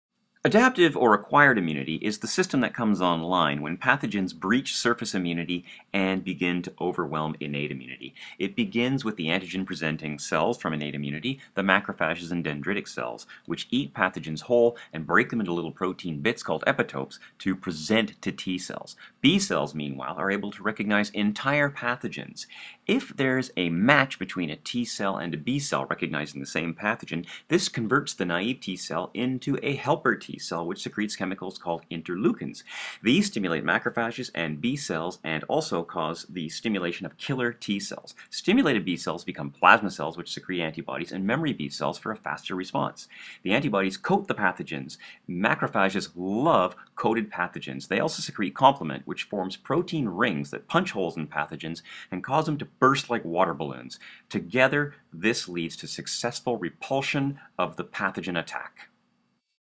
Adaptive Immunity: How Your Immune System Remembers! - One Minute Medical School
Dr Rob explains the 3rd line of the immune response.